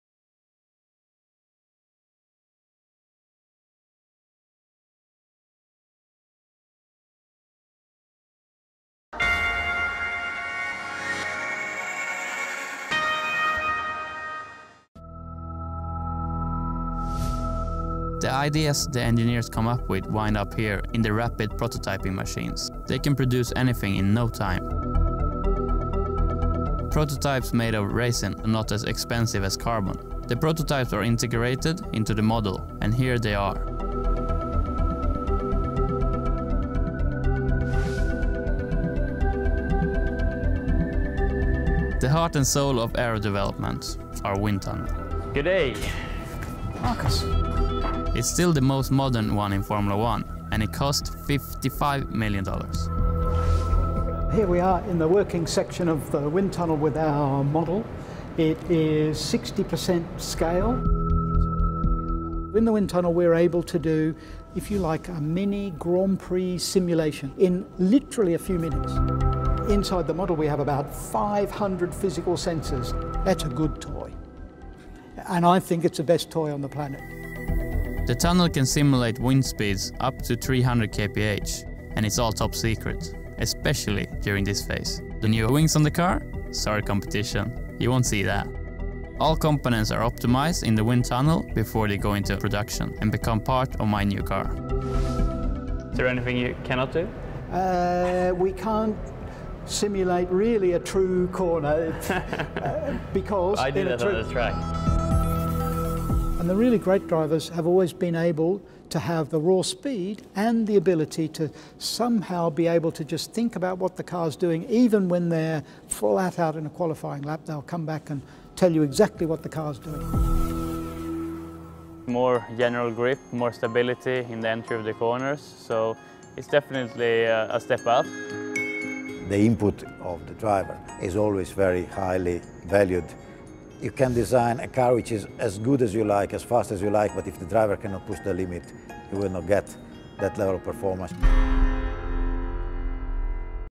FORMULA 1 INSIGHTS - ReSTART: FACTORY VISIT (03/10) - Sauber F1 Team documentary
Marcus Ericsson takes you behind the scenes of the Sauber F1 Team factory in Hinwil, Switzerland. He follows the process of parts being "printed" on our rapid protoyping machines to the wind tunnel test section, where such parts make up most of the 60& model.

Remark: This video was recorded in January 2015 and contains scenes with our former Head of Aerodynamics, Willem Toet, who left our team in March. Willem and our team parted in good spirits and Willem confirmed that he doesn't object to the publishing of this video at all.